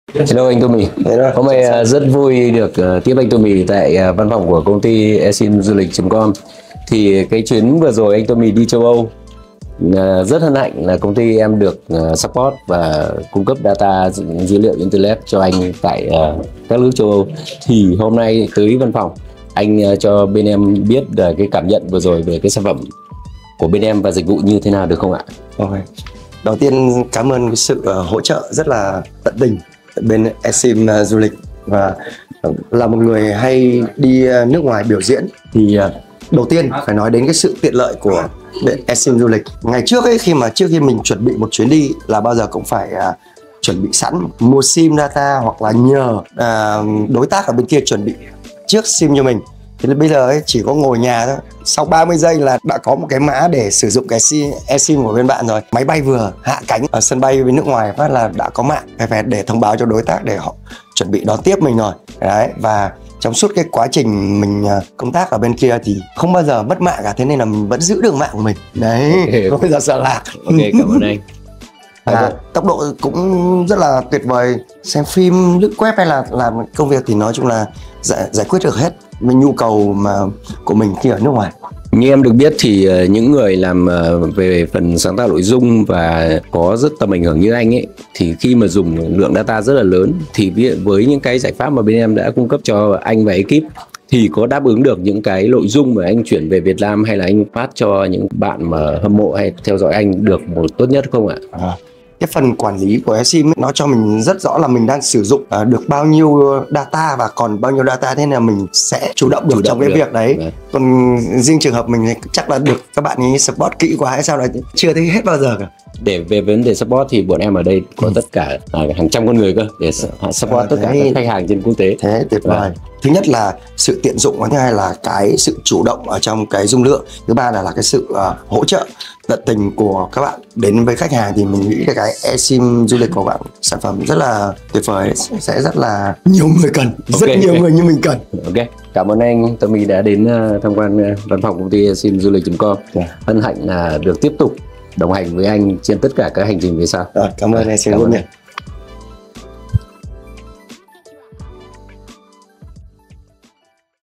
DJ Tommy đã thử và cảm nhận - eSIM Du Lịch
Sau hành trình lưu diễn dài ngày tại Châu Âu, DJ Tommy đã ghé thăm văn phòng của eSIM Du Lịch và có những chia sẻ rất chân thực về trải nghiệm sử dụng eSIM Du Lịch trong chuyến đi lần này. Hãy cùng eSIM Du Lịch trò chuyện với DJ Tommy nhé!

eSIM Du Lịch,
eSIM Travel,
eSIM Global,
eSIM
SIM Du Lịch,